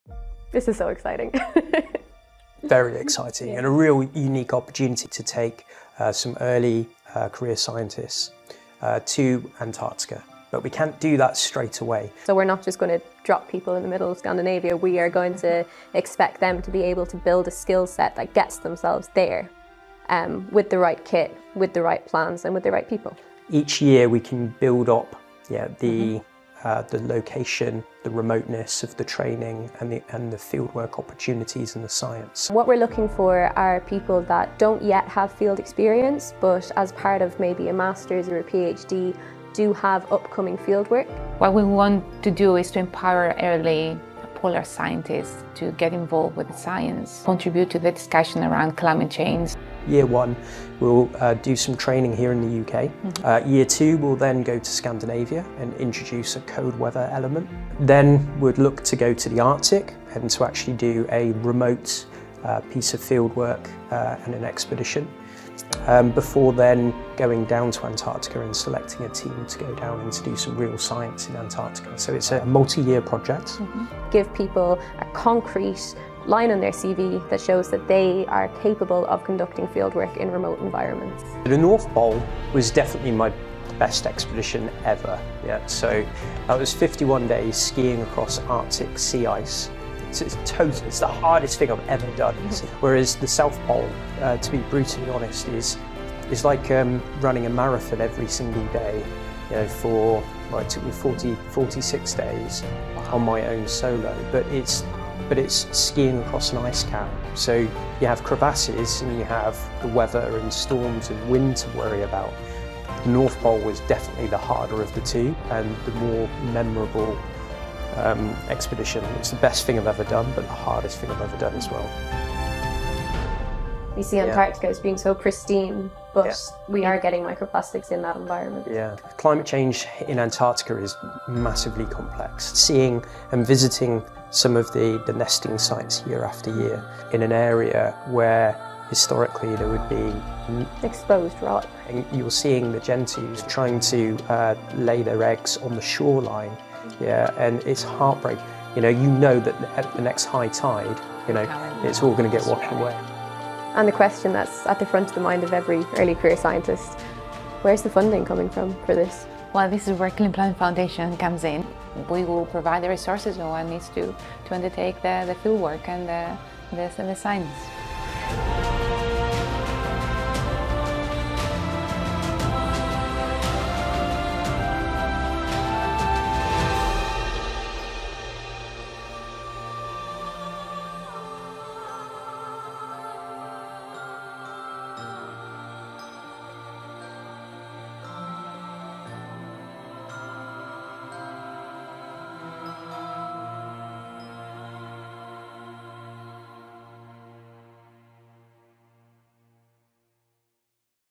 Clean Planet Peninsula - Trailer 2023
A 3 minute trailer to the Clean Planet Peninsula project - watch the full introduction video

The Clean Planet Foundation has launched the Clean Planet Peninsula project in collaboration with polar adventurer, Antony Jinman Ed.D, and the UK Polar Network. The initiative aims to provide early career polar scientists with fieldwork experience whilst raising awareness of environmental challenges in the polar regions, particularly plastic pollution and climate change.

The project offers cost-effective training in the UK and the polar regions through courses, remote fieldwork camps, and expeditions. Participants will gain valuable experience studying environmental challenges and contribute data to the scientific community.